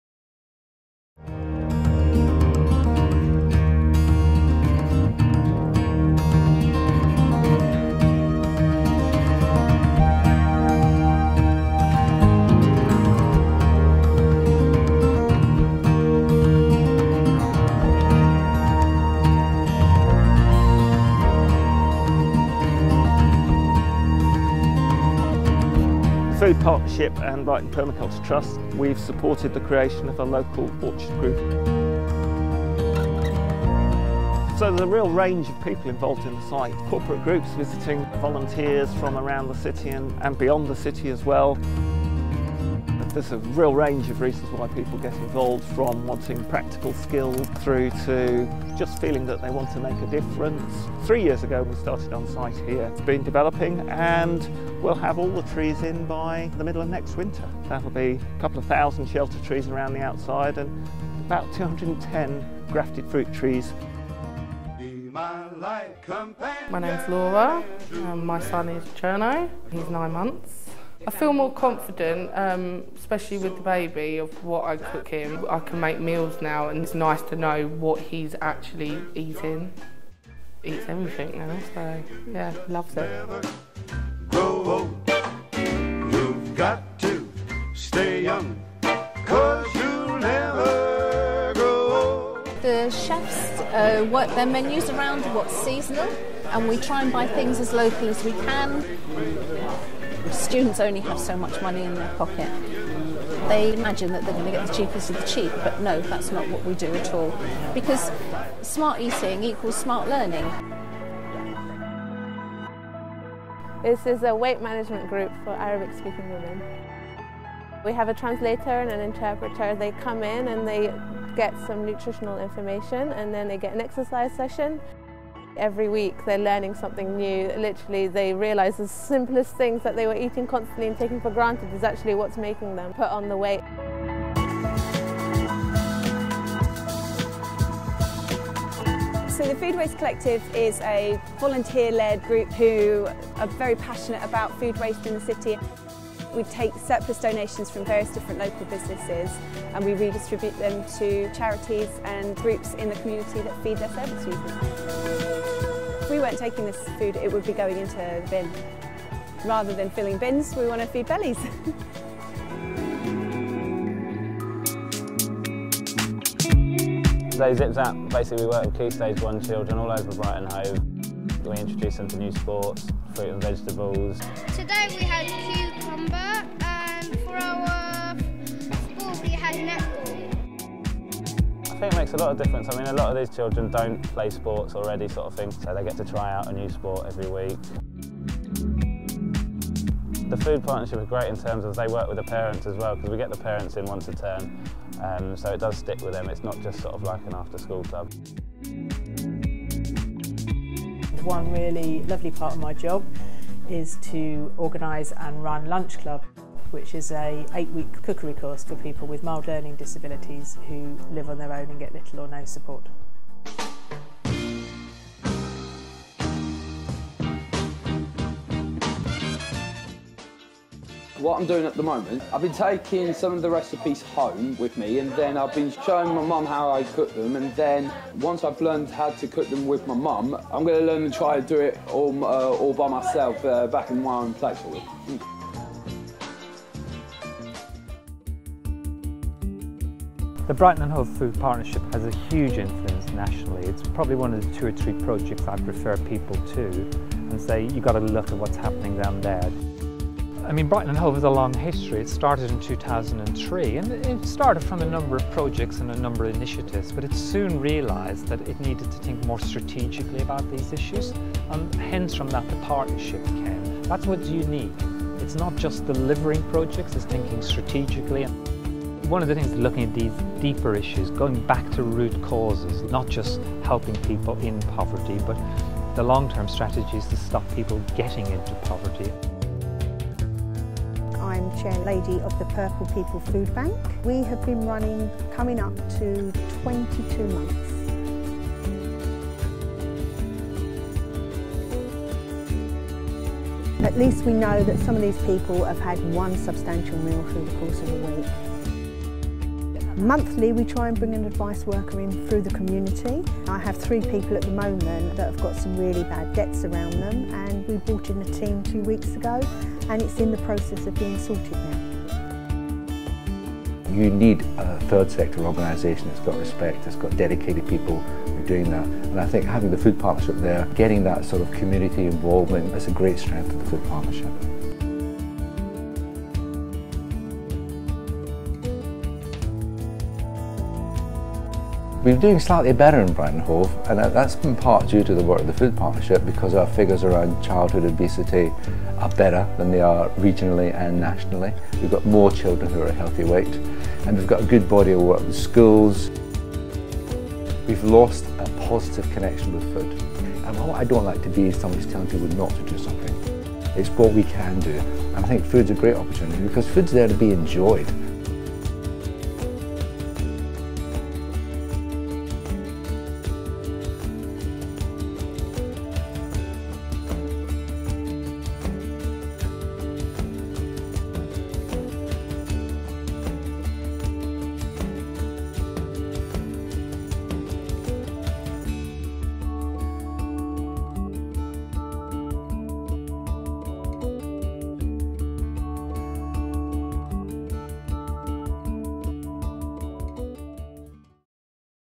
About Brighton & Hove Food Partnership
The Food Partnership is all about growing, cooking, eating (& wasting less) food! Find out about the projects we run in Brighton & Hove and why other cities are looking to replicate this approach.